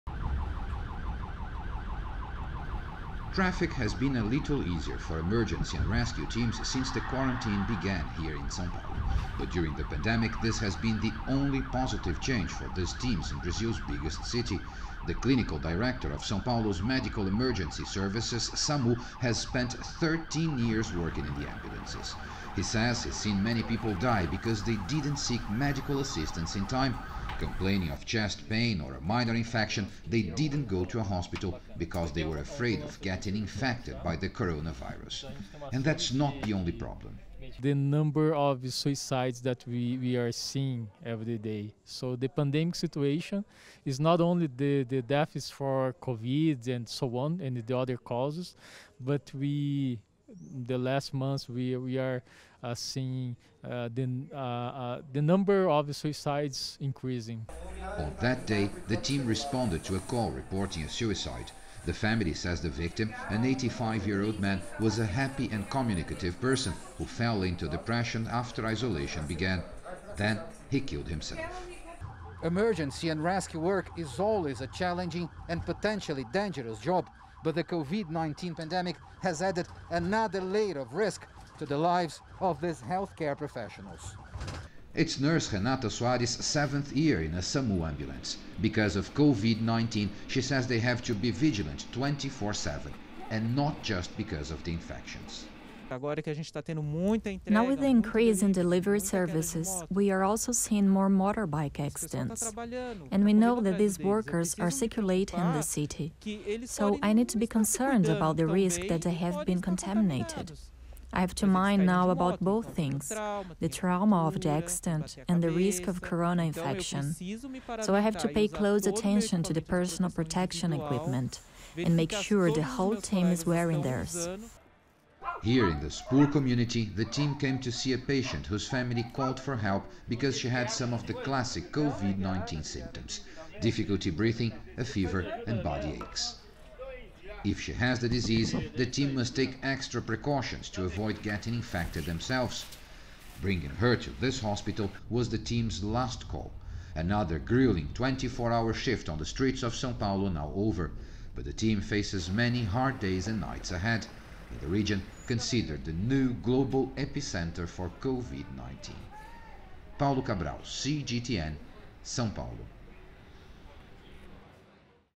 A day in the life of a paramedic in Brazil
Brazil's biggest city Sao Paulo has a total of 120 ambulances for medical emergencies to service of its population of roughly 12 million people.

These health care professionals have a firsthand view of the hardships that people have faced during the pandemic - with problems extending far beyond the coronavirus itself.

CGTN's Paulo Cabral spent the day with a team from Brazil's mobile emergency care service, called SAMU, in Sao Paulo.